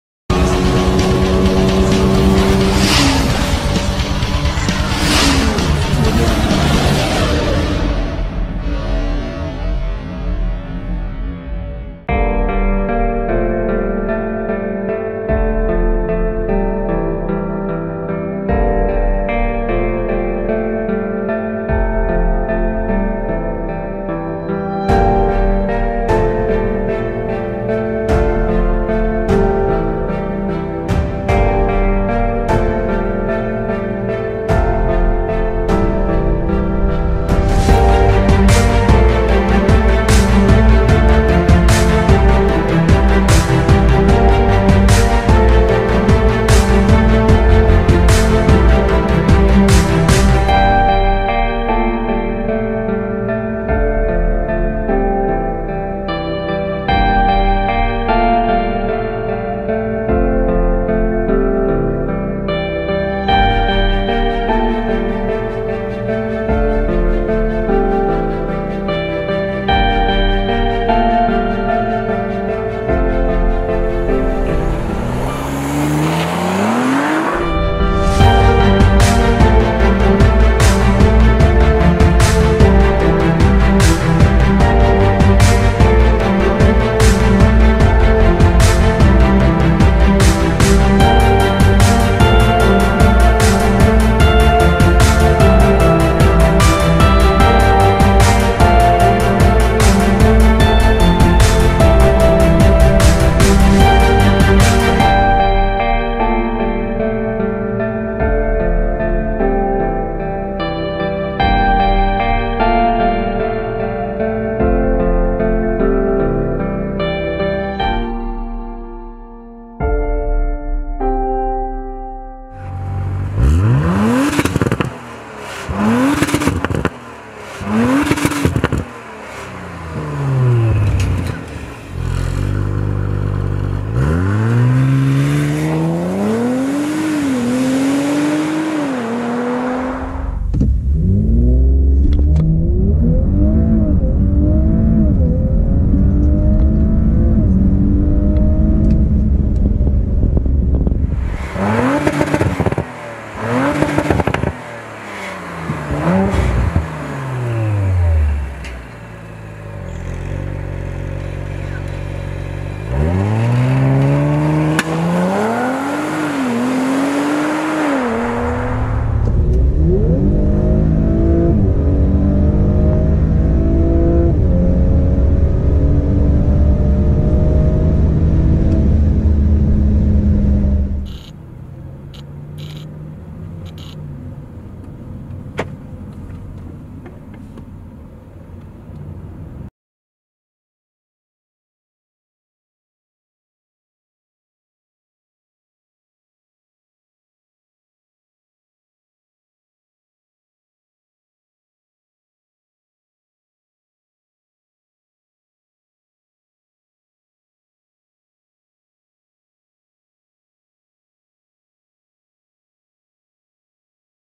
The NEW 2018 AUDI RS5 Coupé
To follow up the success of our video of the misano red 2018 Audi RS5 Coupé, here is a Daytona gray pearl effect one, with black optics and the optional sports exhaust.

Car: 2018 Audi RS5 Coupé (450hp, V6 2.9 BiTurbo)
Color: Daytona gray pearl effect
Performance: 0-100kmh 3.67 (tested), 3.9sec (official)
Location: Neckarsulm/Eberstadt, Germany